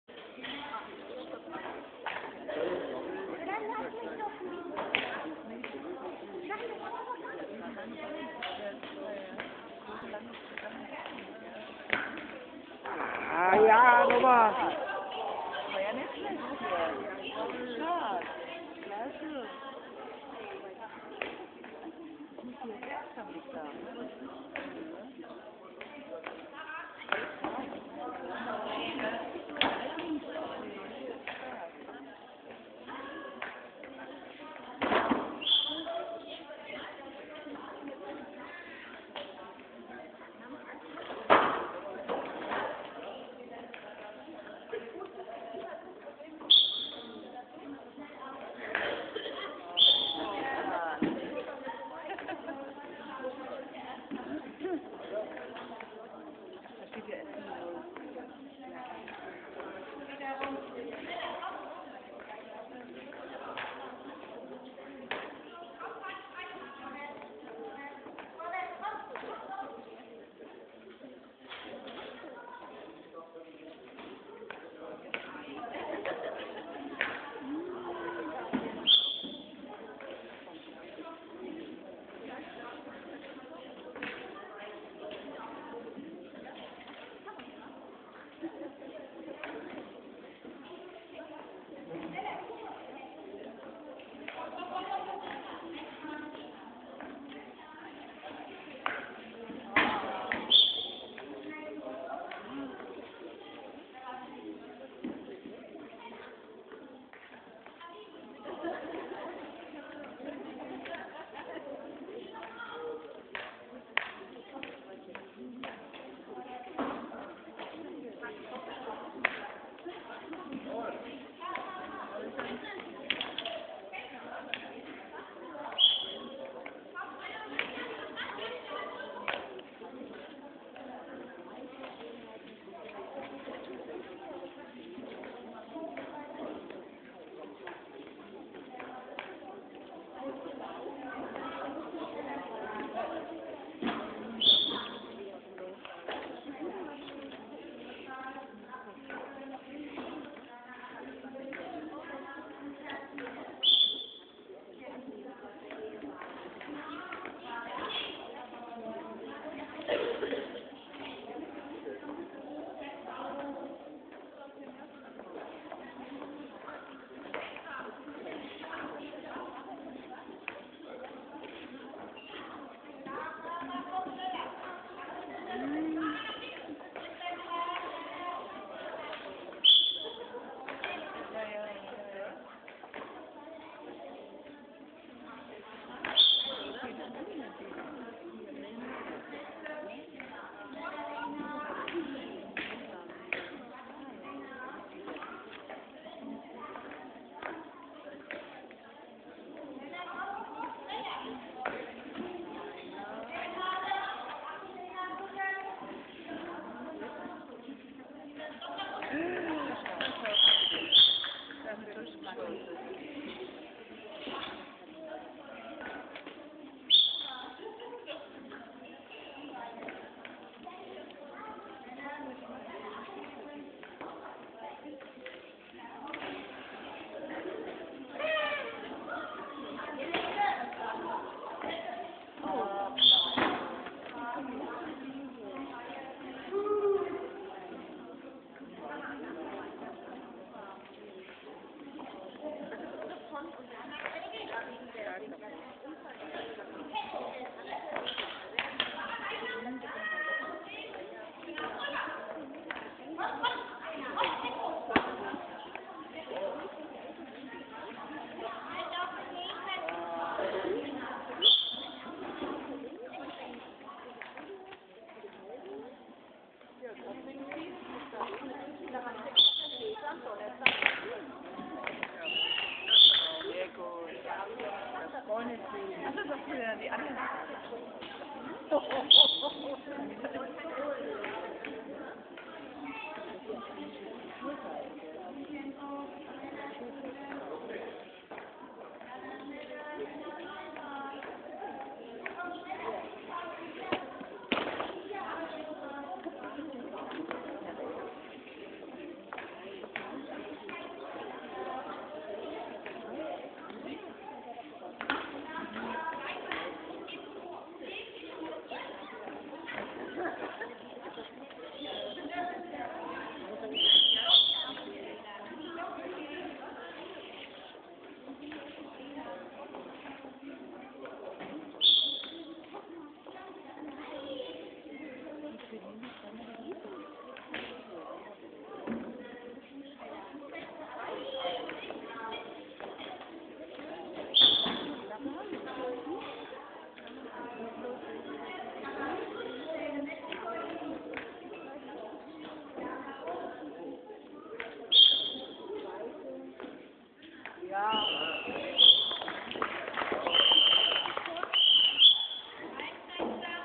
Radpolo U19 Seeheim-Gärtringen
Radpolo Spieltag am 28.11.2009 in Seeheim.
Gärtringen-Seeheim 2
1.Halbzeit